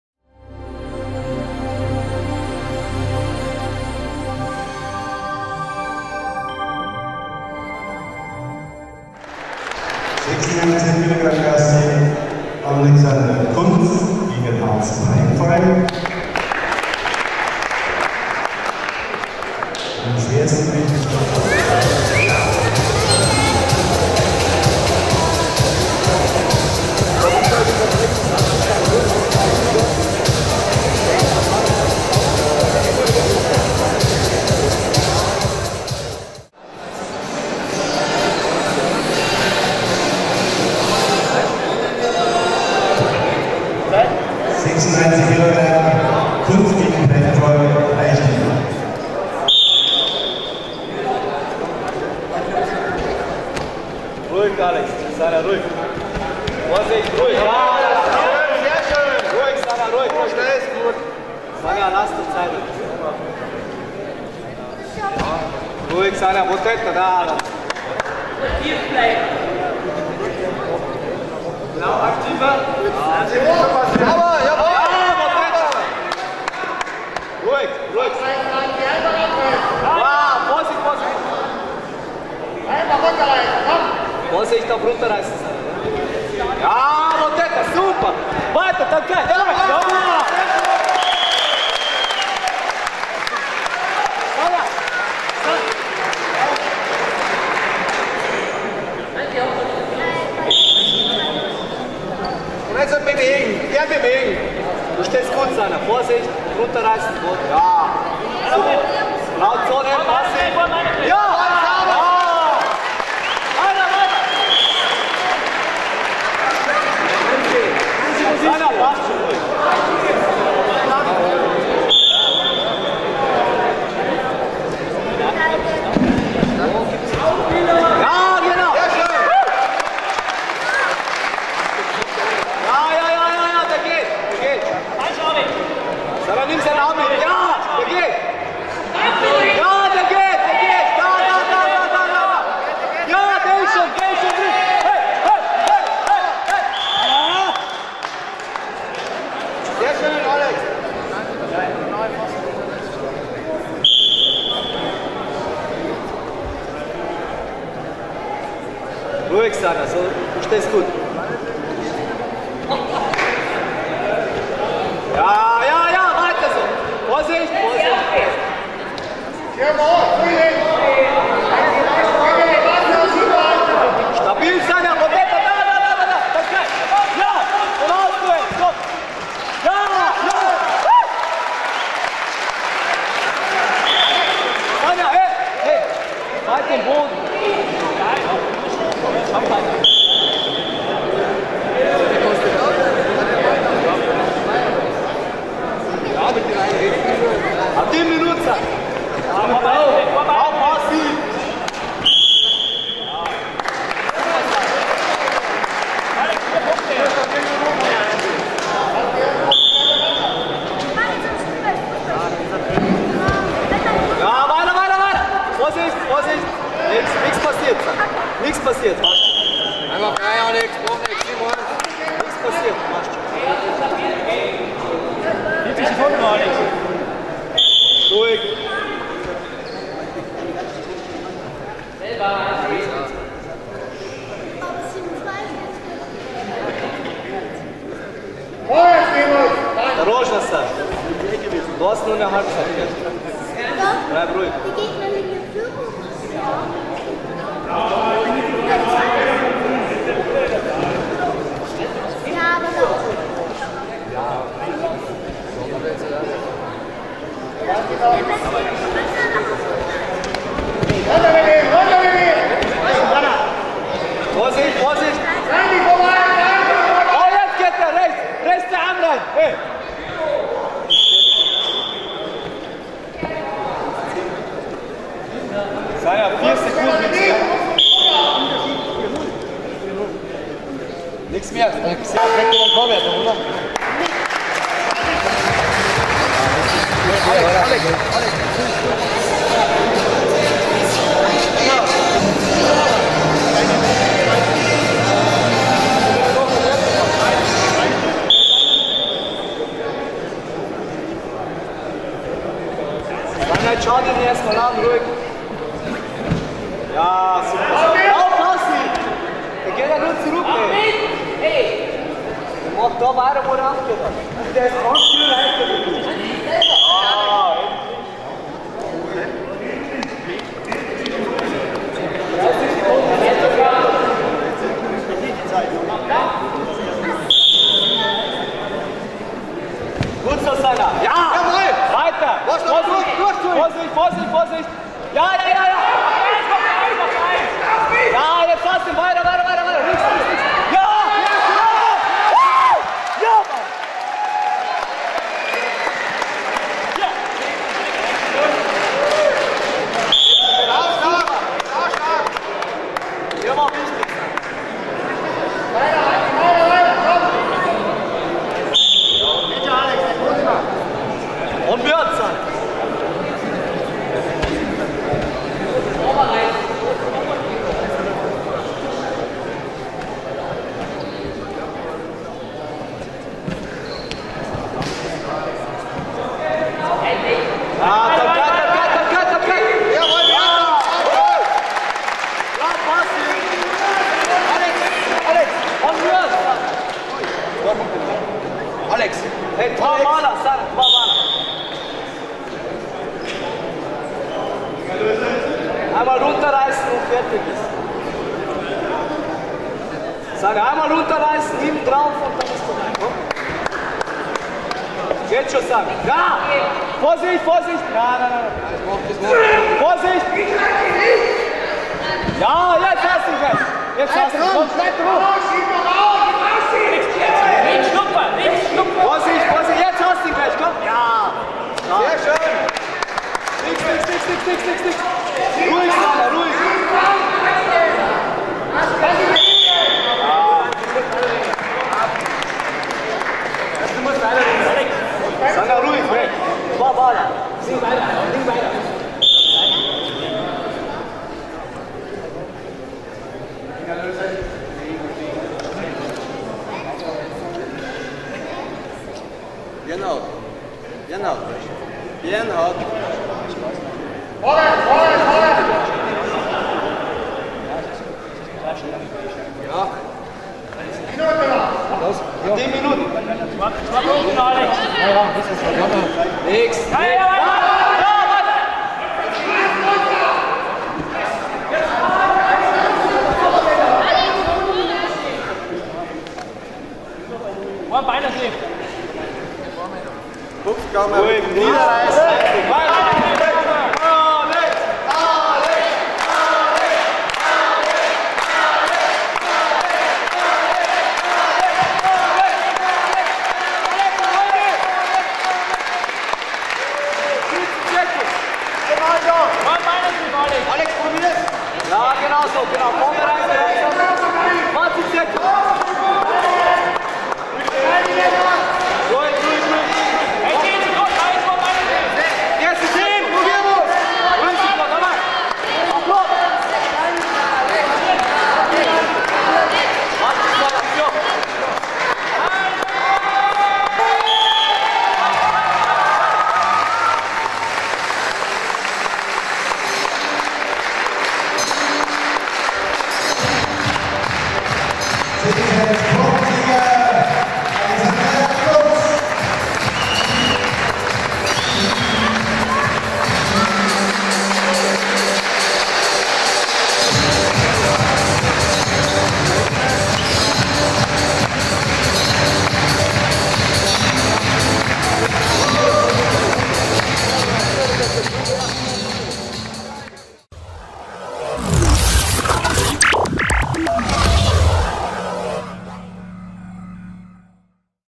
Geiselhöring (Kunz) - Berchtesgaden (Preinfalk) 96kg Freistil (3:1 PS)
6.Kampftag Landesliga Süd (Bayern)
KSA Geiselhöring - TSV Berchtesgaden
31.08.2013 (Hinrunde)